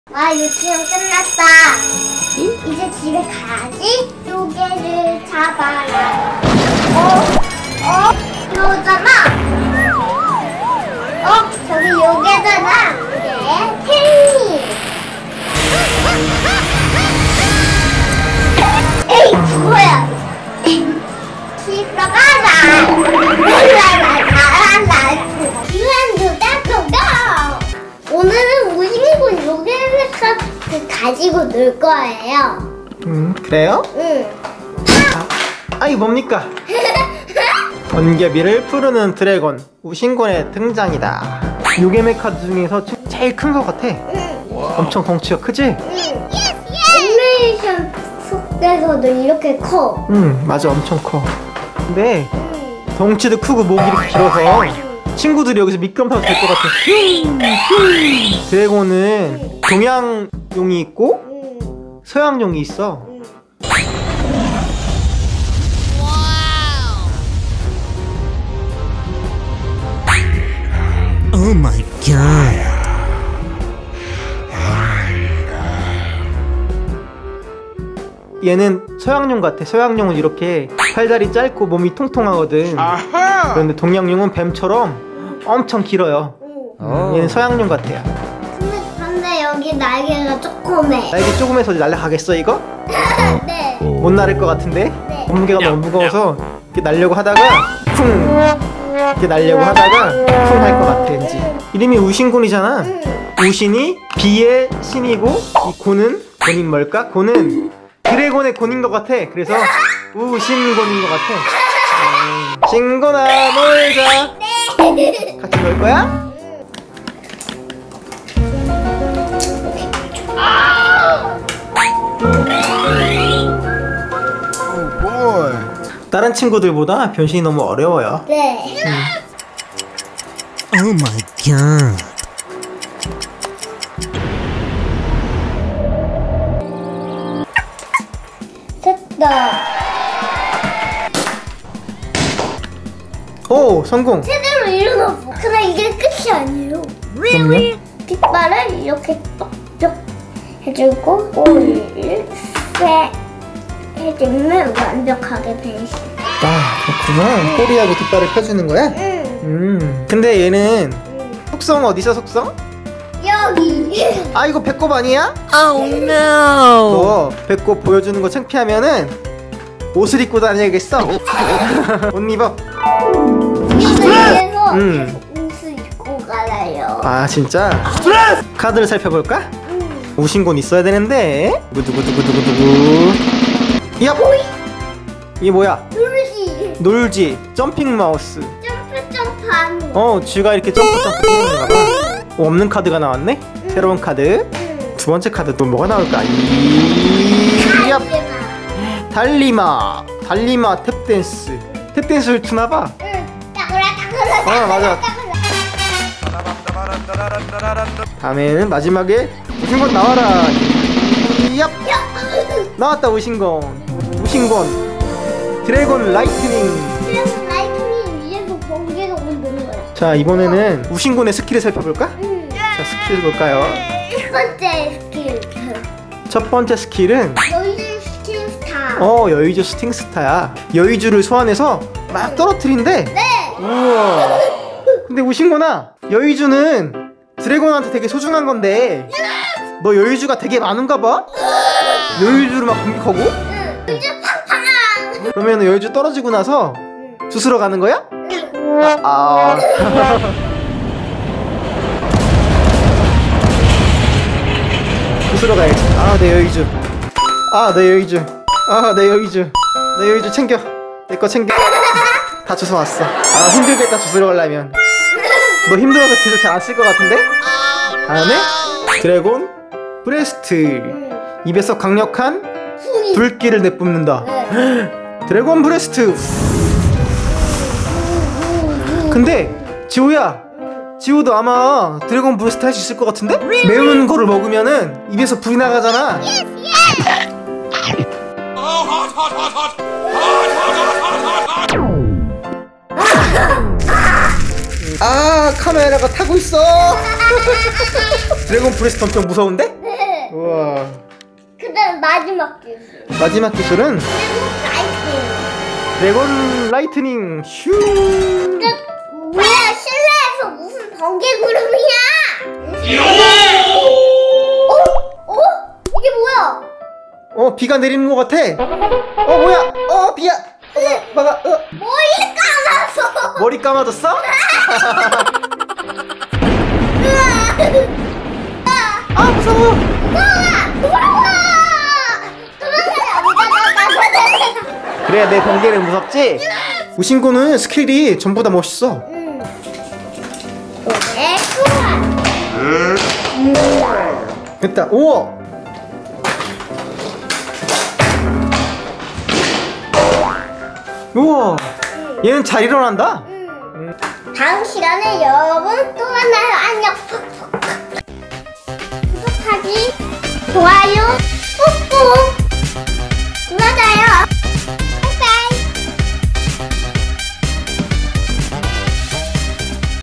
요괴메카드 우신곤! 최강 정령 번개비 우신곤을 속속들이 파헤쳐본다! 용 요괴, 드래곤, Ghost Mecard
정령들의 신이라고 하는 최강의 정령 우신곤을 만나봐요!!
비와 번개를 마음대로 조정한다는데 과연 소문만큼 강한 정령일까요?
어떤 정령일지 같이 파헤져 볼까요?

Let's play with ghostmecard